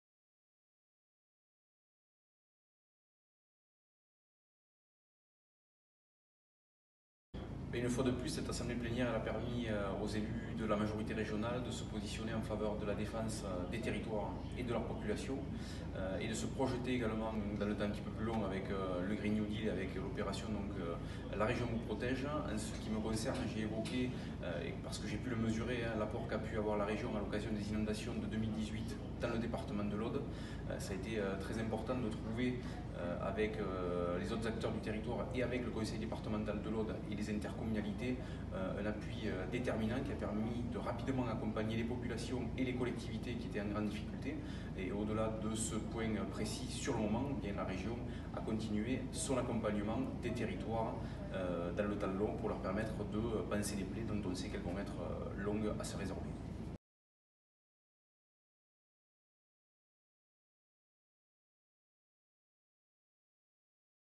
L'Occitanie protège - retour sur l'action de la Région Occitanie suite aux inondations dans l'Aude
A l'occasion de l'adoption par la Région du rapport L'Occitanie Protège, Philippe Andrieu, conseiller régional d'Occitanie, rappelle l'importance de la présence de la Région suite aux inondations dans l'Aude.
"La Région Occitanie est une Région qui est présente pour accompagner les territoires et les populations au quotidien dans la crise"